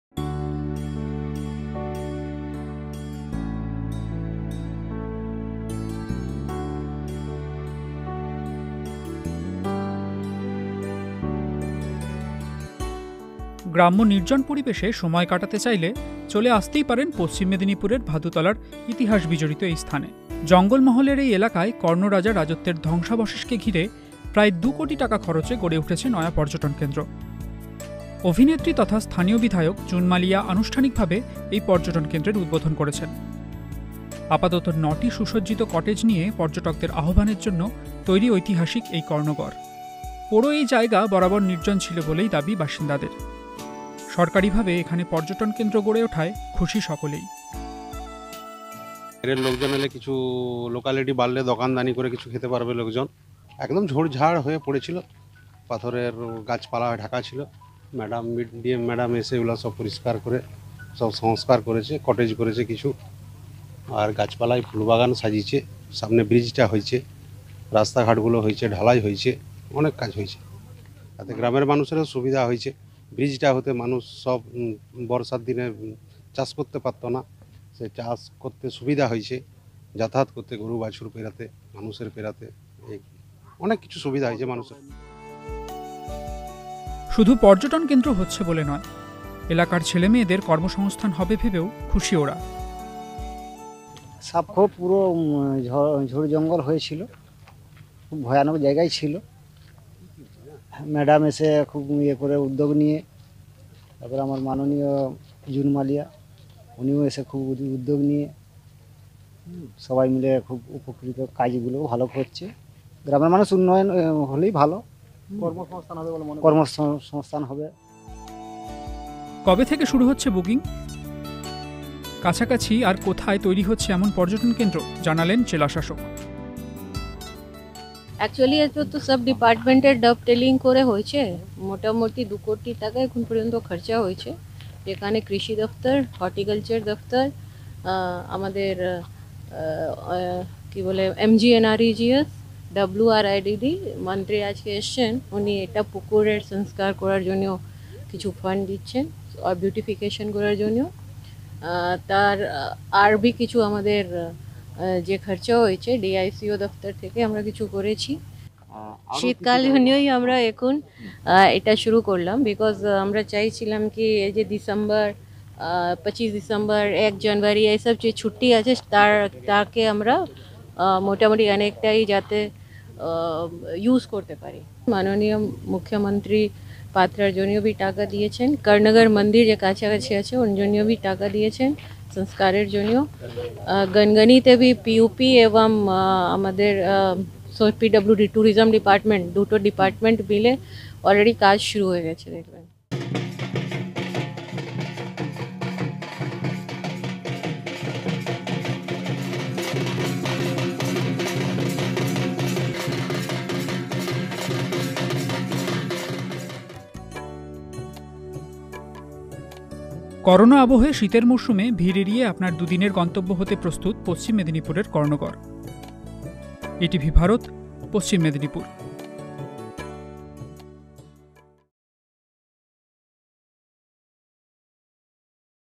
Tourist Spot in West Medinipur : 2 কোটি টাকা ব্যয়ে পশ্চিম মেদিনীপুরে তৈরি নয়া পর্যটন কেন্দ্র
গ্রাম্য নির্জন পরিবেশে সময় কাটাতে চাইলে চলে আসতে পারেন পশ্চিম মেদিনীপুরের ভাদুতলার ইতিহাস বিজড়িত এই স্থানে (Tourist Spot in West Medinipur) ৷ আপাতত 9টি সুসজ্জিত কটেজ নিয়ে পর্যটকদের আহ্বানের জন্য তৈরি ঐতিহাসিক এই কর্ণগড় ৷